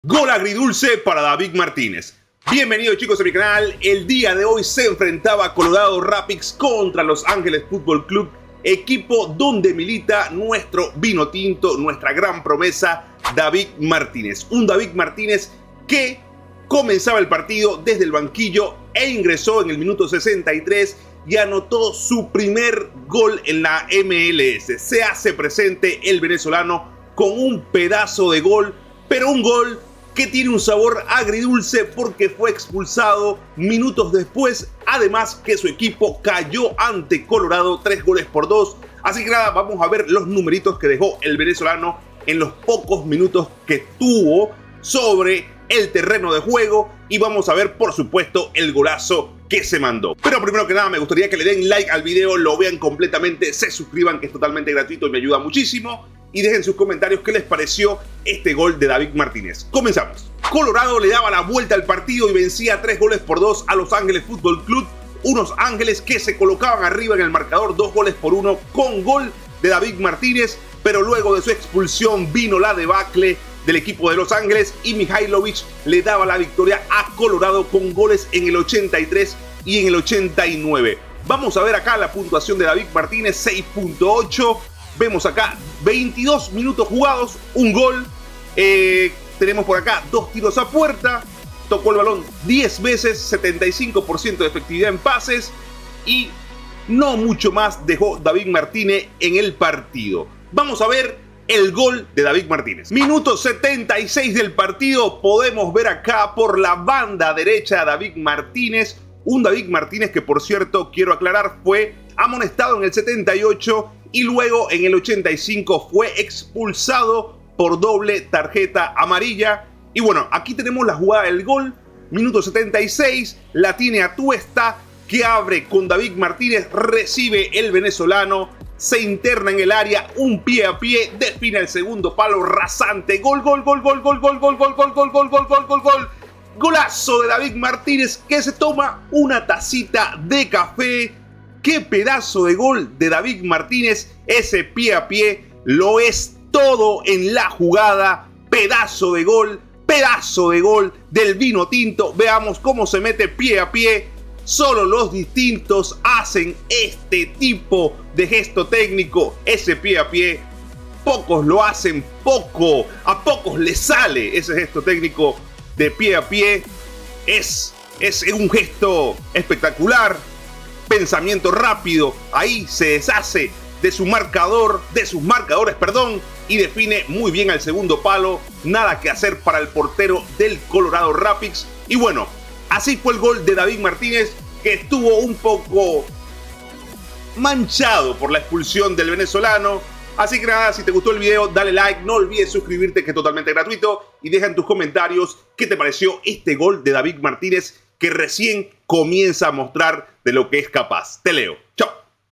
GOLAZO del VINOTINTO DAVID MARTÍNEZ en la MLS | PRIMER GOL y EXPULSIÓN de DAVID MARTÍNEZ
Juntos, podemos lograr grandes cosas. Déjame saber si te gusta el contenido del canal y comenta que contenido te gustaría ver.